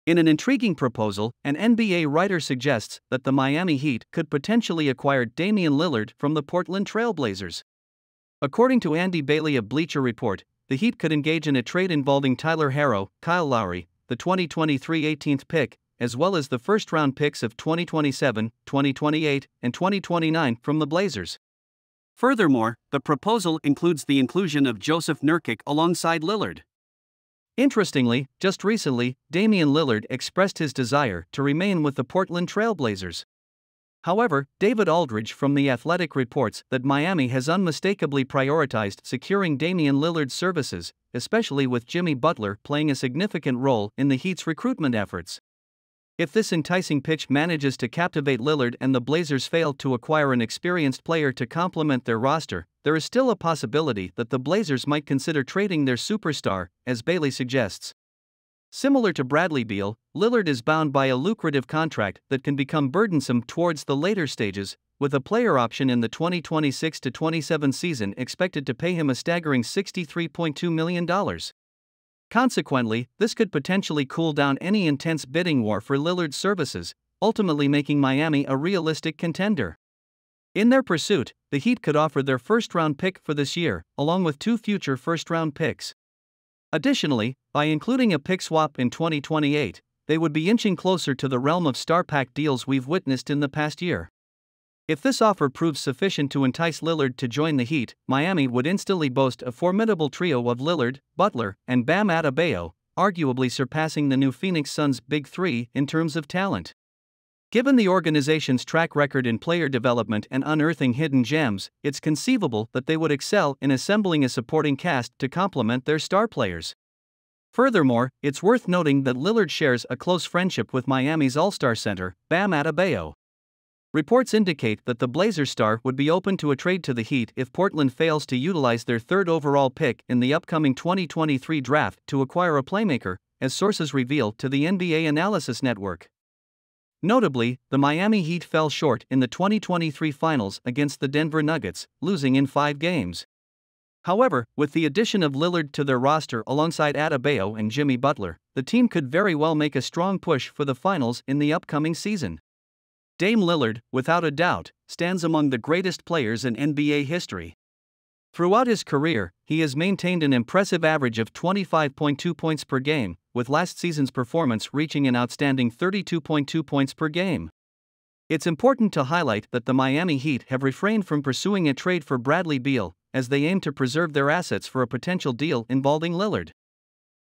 "Miami Heat's Game-Changing Move: Acquiring Damian Lillard? | NBA Trade Rumors"
In this video we’ll walk you through:
- ( Introduction: "Could the Miami Heat be on the brink of a game-changing trade? In this video, we explore the intriguing proposal that suggests the Heat could acquire Damian Lillard from the Portland Trail Blazers. Stick around as we dive into the details and analyze the potential impact on both teams!"  )

Our channel is about nba news. We cover lots of cool stuff such as (nba news), (lakers news) and (knicks news)